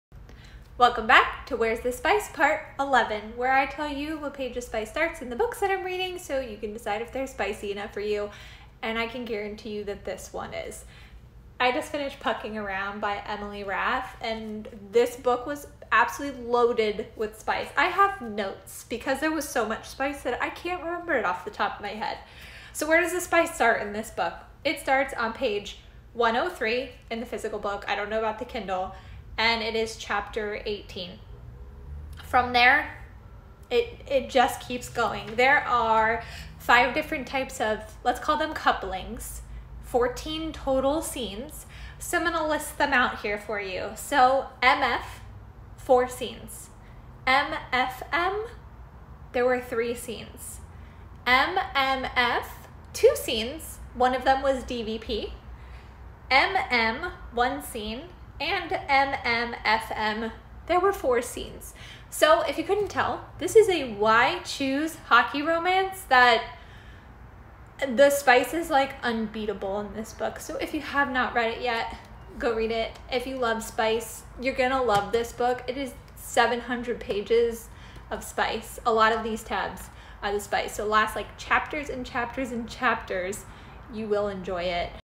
Where’s the spice? Pt. 11 - Pucking Around 🌶️ ​⁠booktube
Where’s the Spice? Pt. 11 - Pucking Around by Emily Rath 🌶️